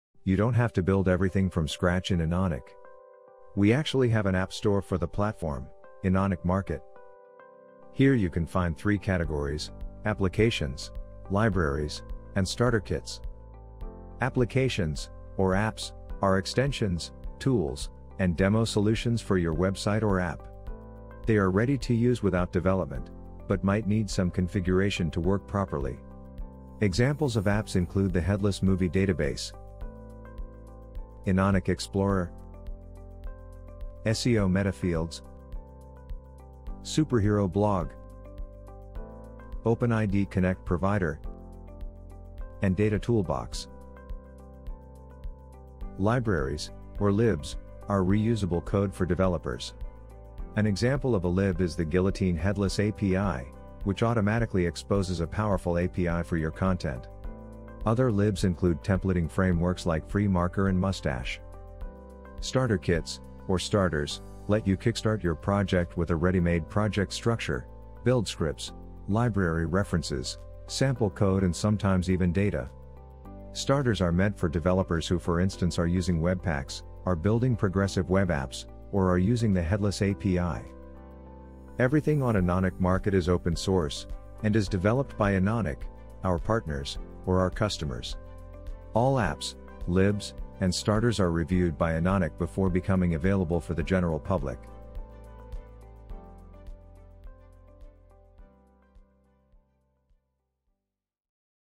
What Is Enonic Market? [Admin in Enonic]
Enonic XP version 7.8.0, Content Studio version 4.0.0

You don’t have to build everything from scratch in Enonic. We actually have an app store for the platform: Enonic Market.

Here you can find three categories: Applications, libraries, and starter kits.
Applications, or apps, are extensions, tools, and demo solutions for your website or app. They are ready to use without development, but might need some configuration to work properly. Examples of apps include the Headless Movie Database, Enonic Explorer, SEO meta fields, Superhero Blog, OpenID Connect provider, and Data Toolbox.

Libraries, or libs, are reusable code for developers. An example of a lib is the Guillotine Headless API, which automatically exposes a powerful API for your content. Other libs include templating frameworks like FreeMarker and Mustache.

Starter kits, or starters, let you kickstart your project with a ready-made project structure, build scripts, library references, sample code and sometimes even data. Starters are meant for developers who for instance are using webpacks, are building progressive web apps, or are using the headless API.

Everything on Enonic Market is open source, and is developed by Enonic, our partners, or our customers. All apps, libs, and starters are reviewed by Enonic before becoming available for the general public.